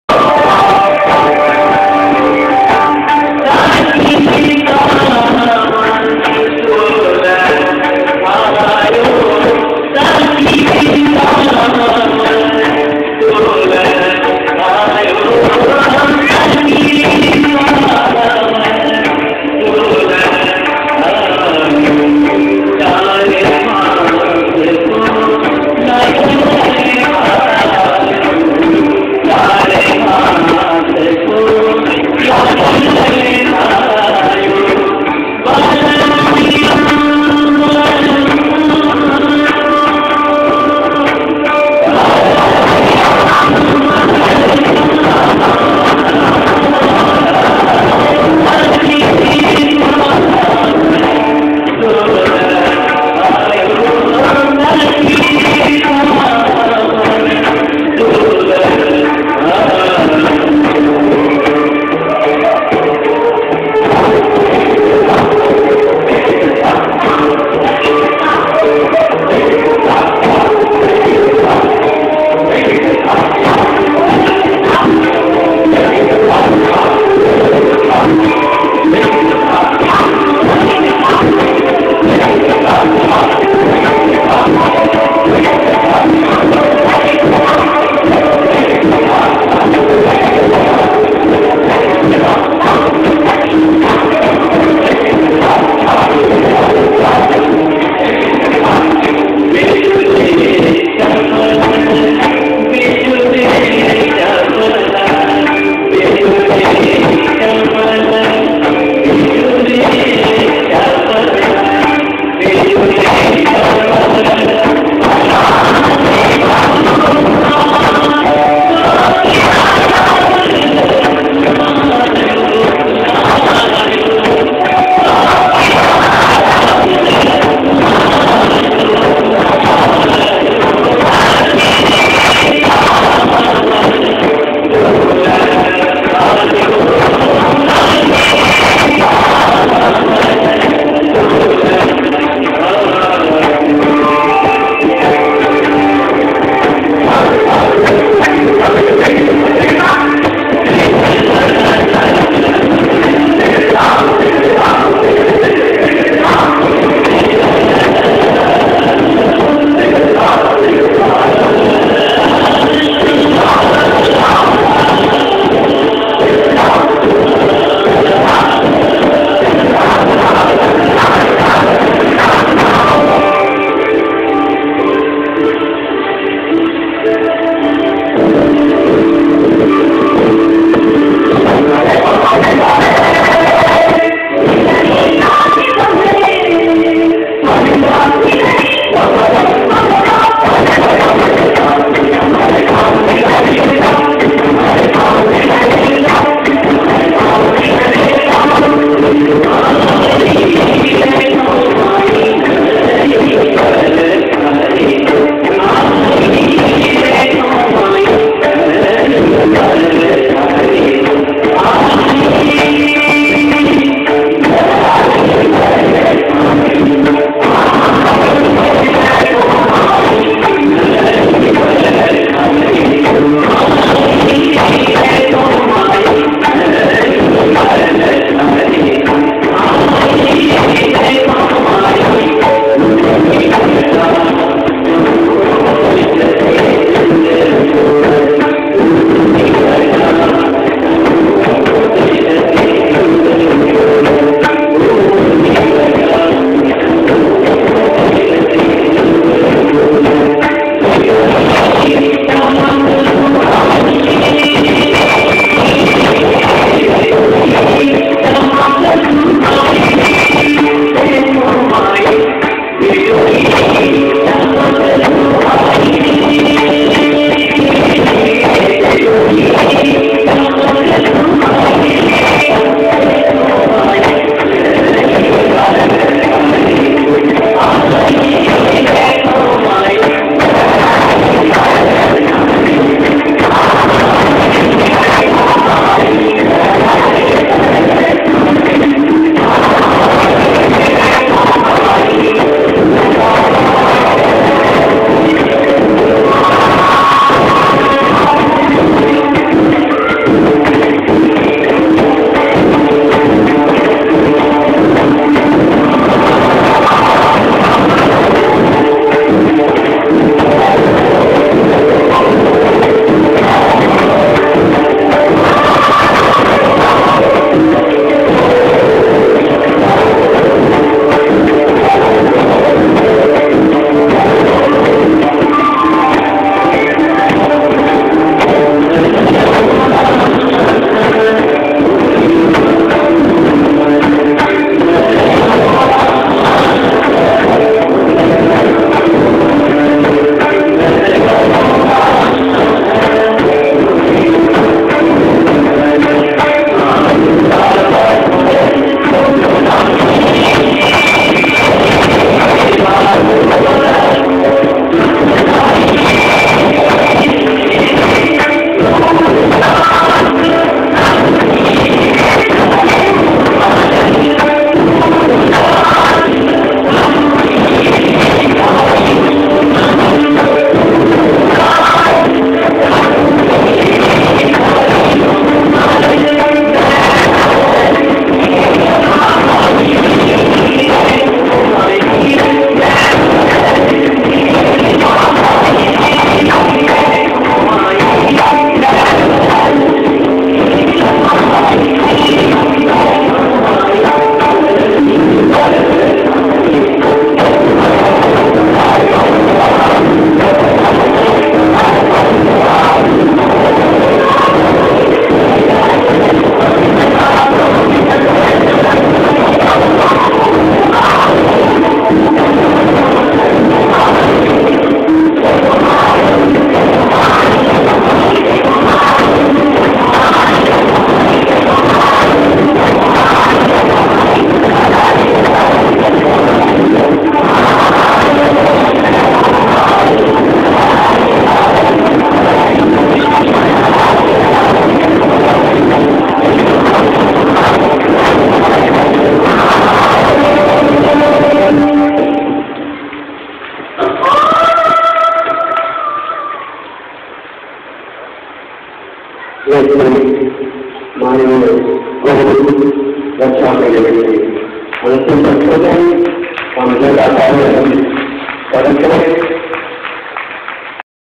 Kathak guru mrs. Raksha Singh David : Part 3
A workshop was organized from 1-June-2015 to 7-June-2015 at Hazaribag.
On the final day she perform in Vivekanand Sabhagar, Vinoba Bhave University, Hazaribag.